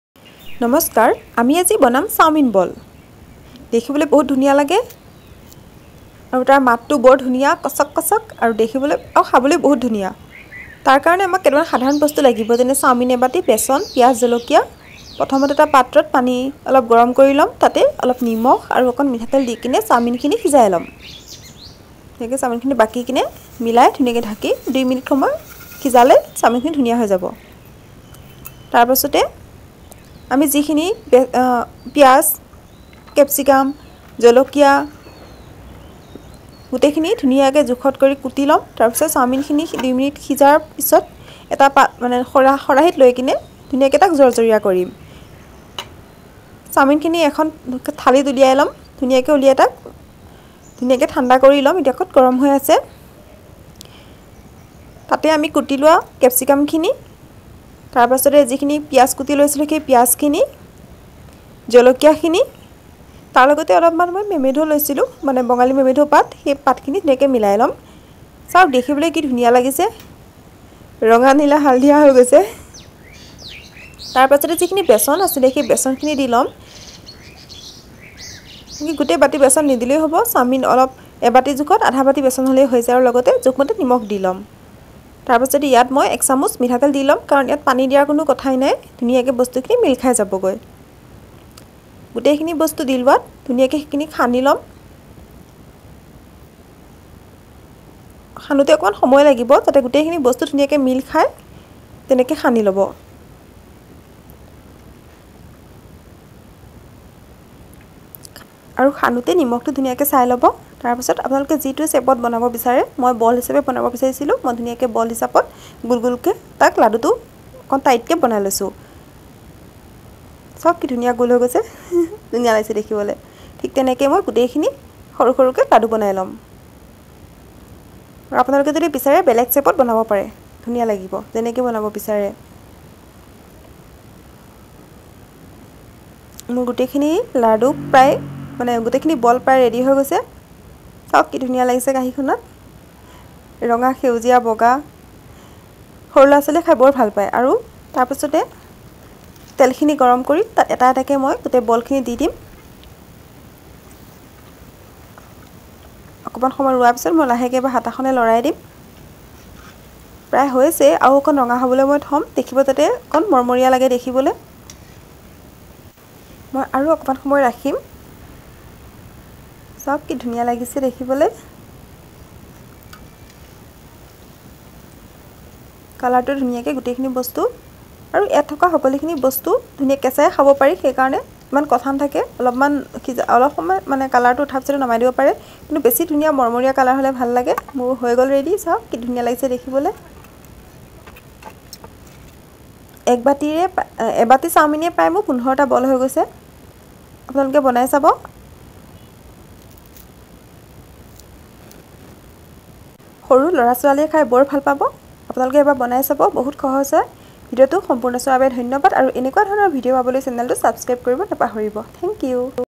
ছাউমীন বল🔥| Easy & Quick Chaumin Ball Recipe | Aneke Chaumin Khai Paise ne🤔| Evening Snacks Recipe
ছাউমীন বল 🔥। Easy & Quick Chaumin Ball Recipe | Aneke Chaumin Khai Paise ne 😋| Evening Snacks Recipe| @VillageCooking-Assam

1000 Mini চিংৰা😱 | Easy & Quick Chamusa Recipe | Homemade Singara Recipe |Testy Evening Snacks |